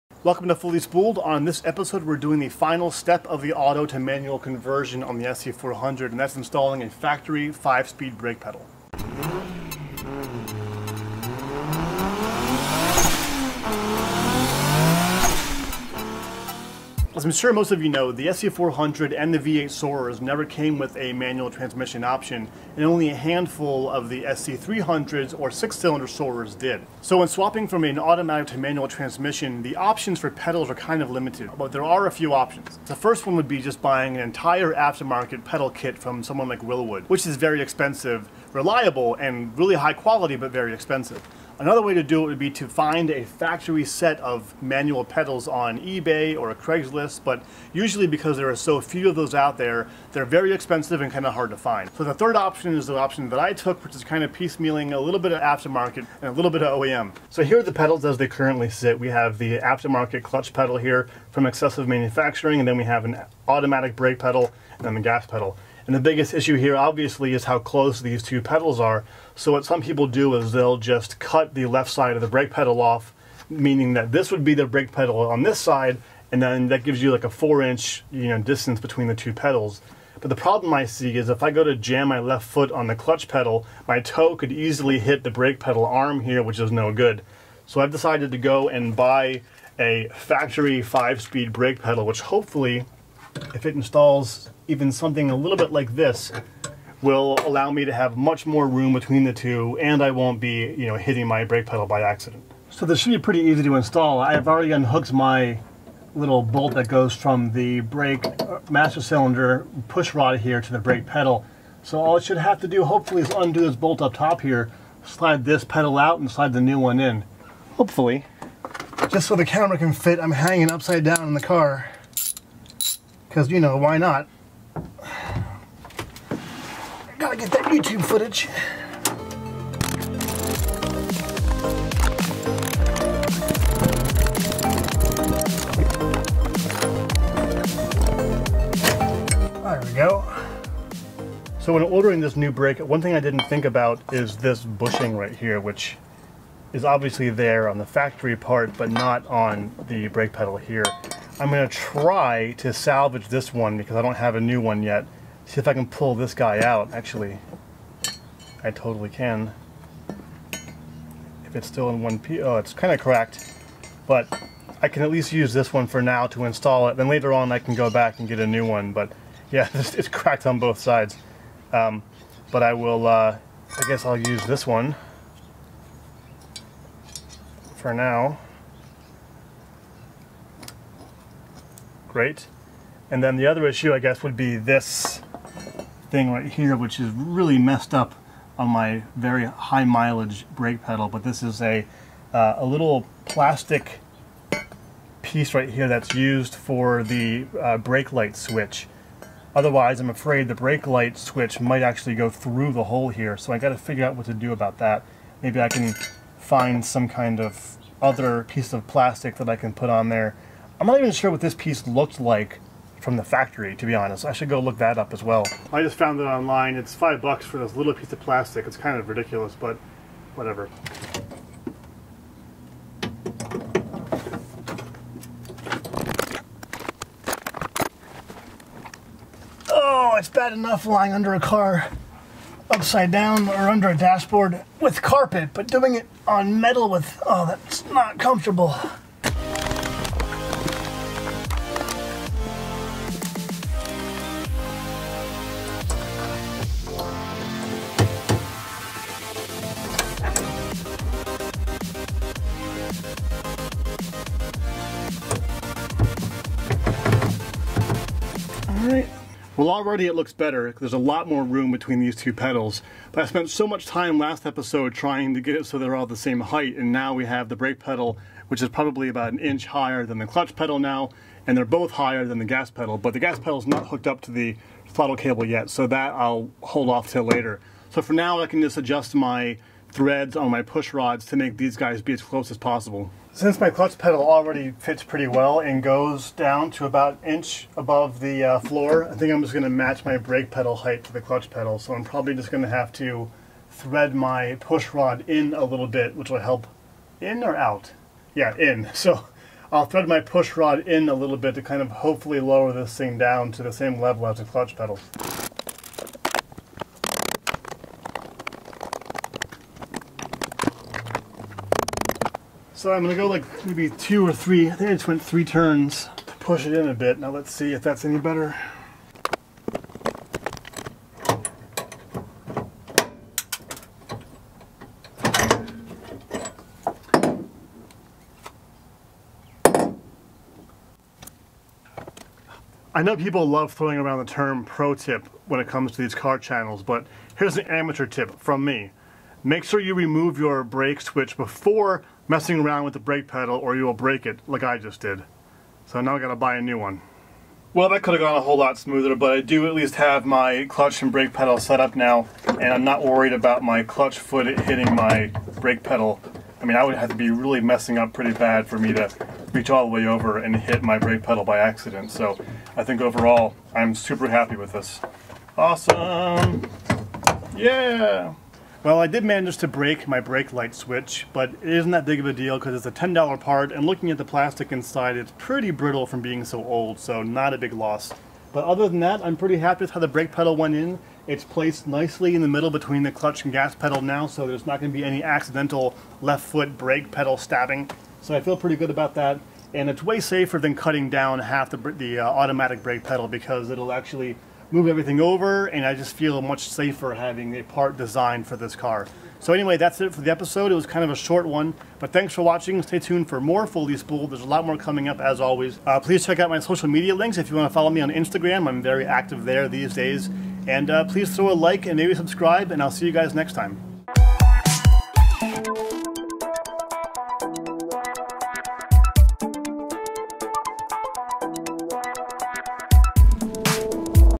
Lexus SC300 Drift Build. Ep.30 - OEM Auto to Manual Brake Pedal Swap
Jeff installs the OEM Lexus brake pedal from a manual 5 speed SC300 into the SC400 to complete the auto to manual swap. It went pretty smoothly except for that parts that didn't.

Thanks!

Part Numbers:

Manual Brake Pedal: Lexus Part No.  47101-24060

Brake Pedal "stop cushion" or "stop pad" (I found a much cheaper one than in the video for only 95 cents. Lexus Part No. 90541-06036

Bushings to replace my cracked ones (need 2): Lexus Part No. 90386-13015

[Creative Commons]
Provided by ARGOFOX